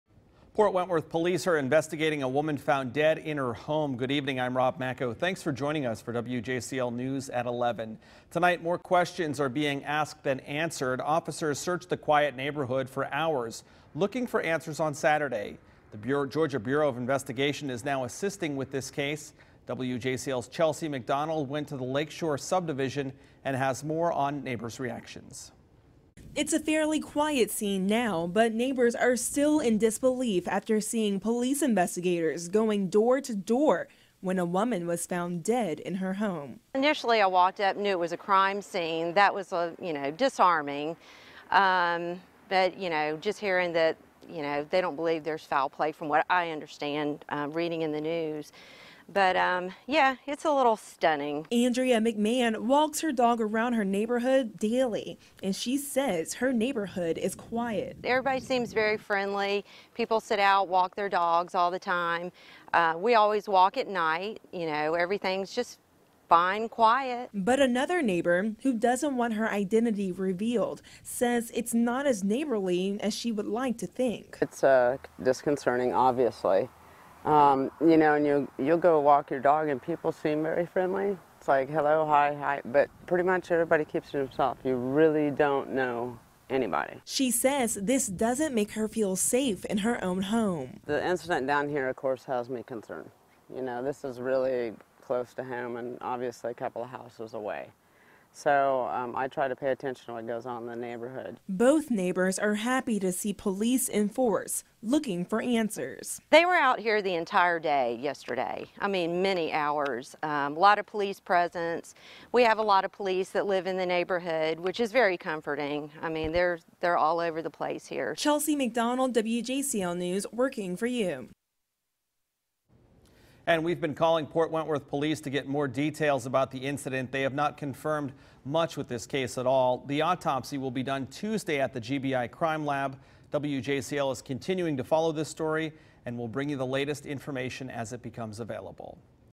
Few details emerge in woman's death
Police continue to investigate death of Port Wentworth woman.